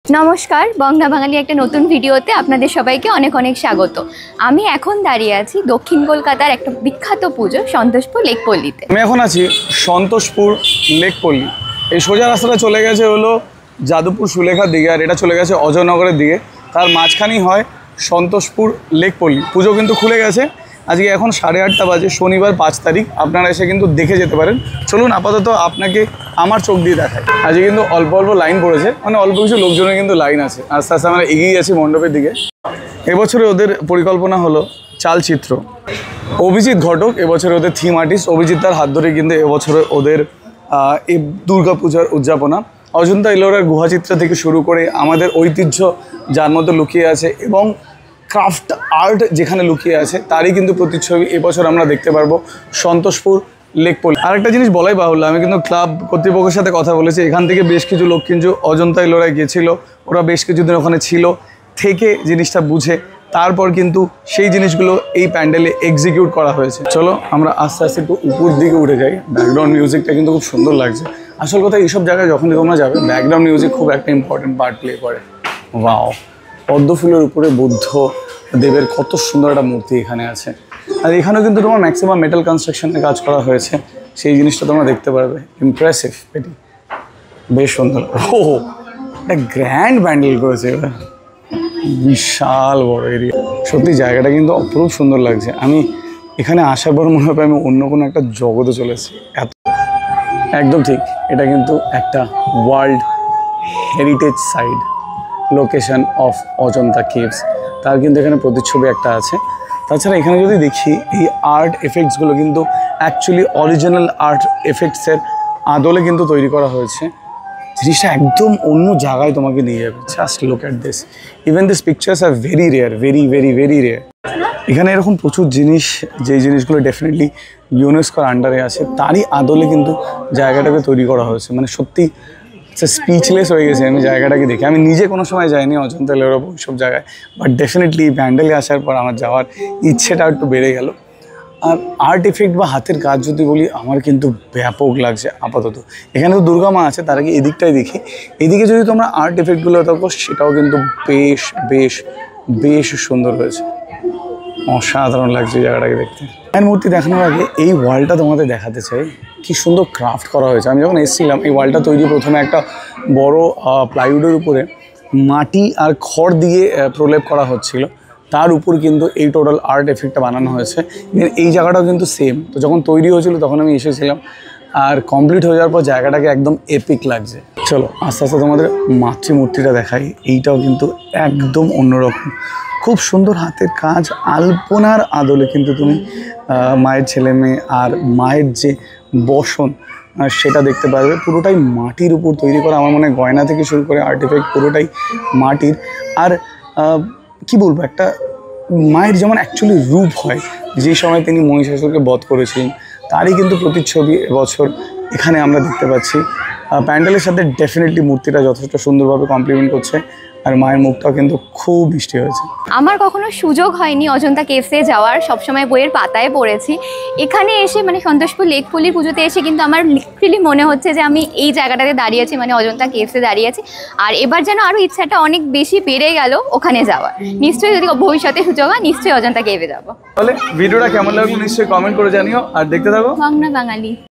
Santoshpur Lake Pally Durga Puja 2024 | দূর্গা পূজা পরিক্রমা ২০২৪
🙏 দুর্গা পূজা পরিক্রমা ২০২৪🙏
সন্তোষপুর লেক পল্লী  দূর্গা পূজা ২০২৪ এই বছর আপনাদের নিয়ে যাবে অজন্তা ইলোরার গুহার একদম ভিতরে।।।